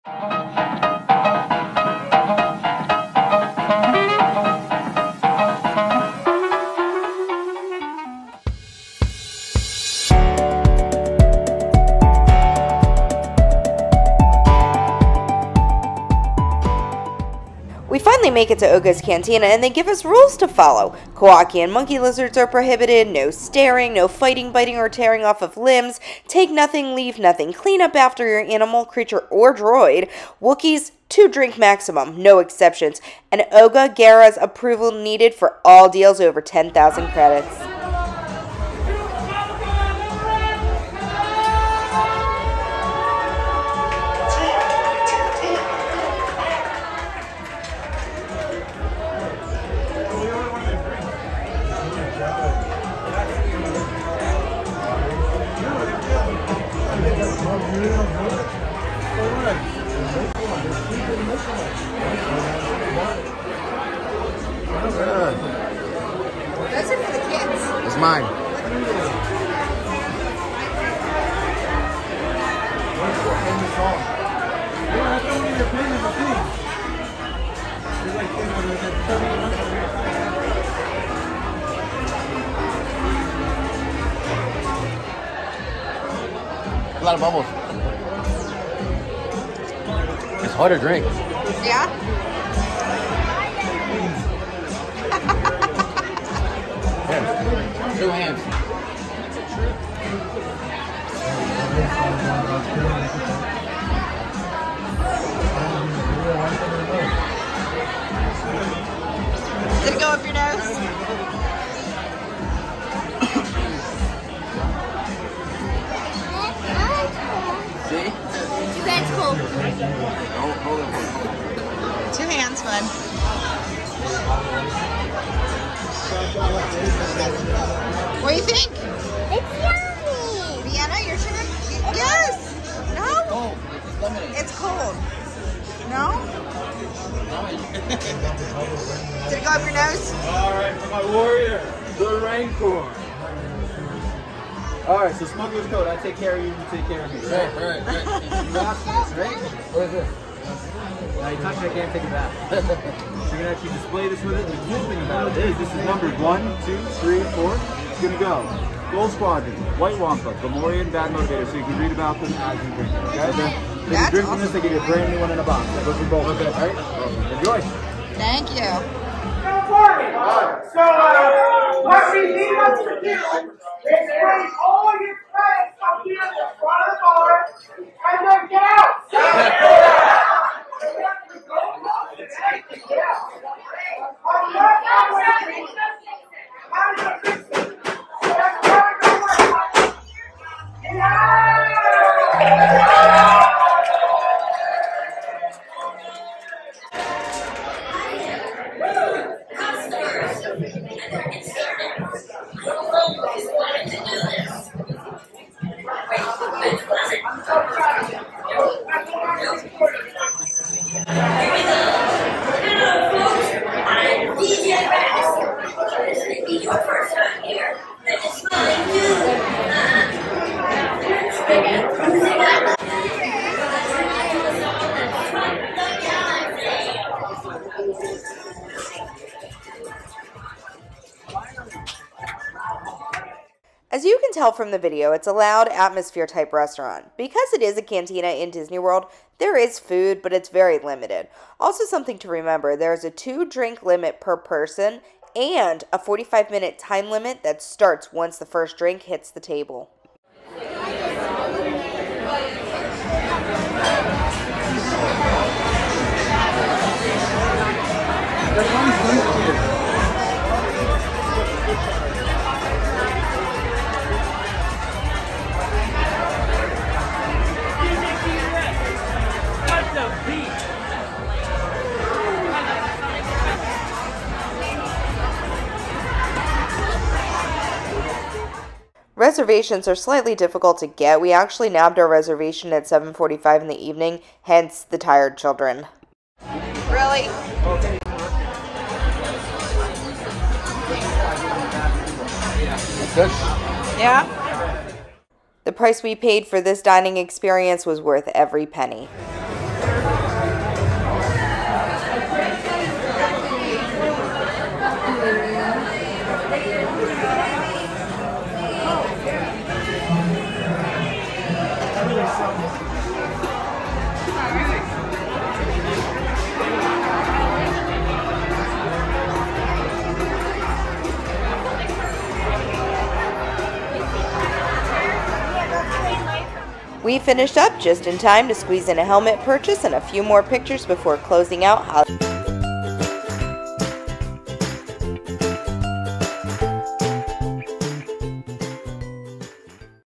Jammin with DJ Rex in Oga's Cantina
We checked out Oga's Cantina for the first time in Hollywood Studios Orlando. Booking our reservation was a bit difficult. We finally grabbed a reservation a tad before 8PM and took the time to wear the children out before getting to there. Check out our drinks and the really cool atmosphere that the Cantina provides!